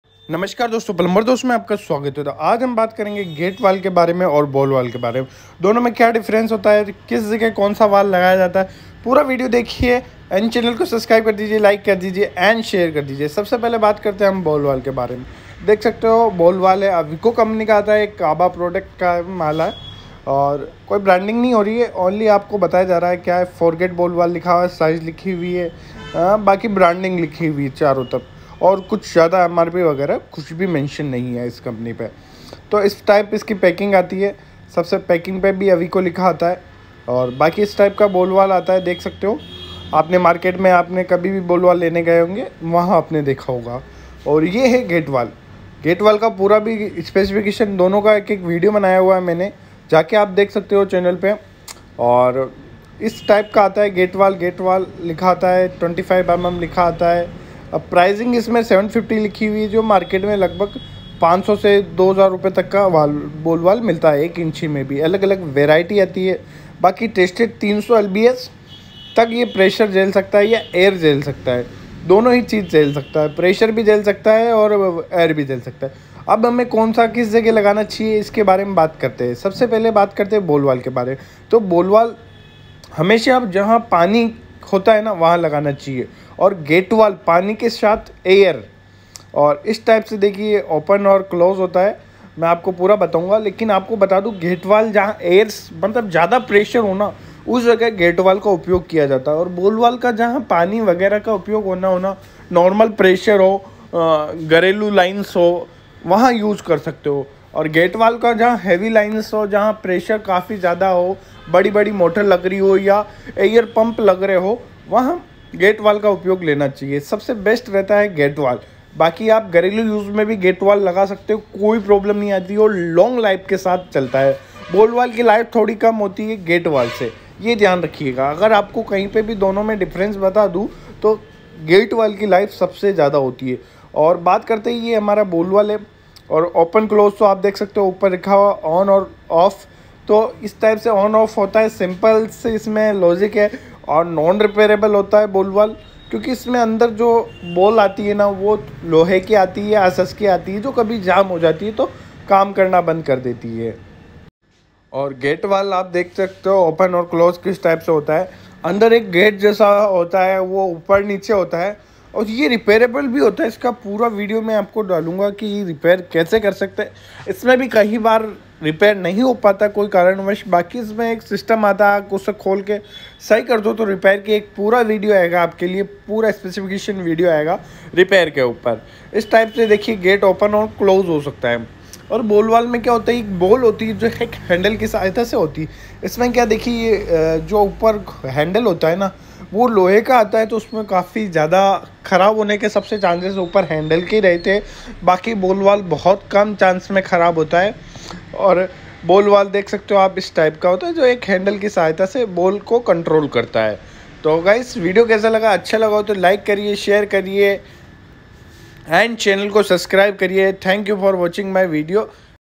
Gate valve or ball valve में क्या diffrance आता है । gate valve vs ball valve full comparison
Namaskar dosto plumber Dost me apka swagt hai aj hum bat krnge ball valve or gate valve me kya diffrace hai hummed hogi apko video pasand ayga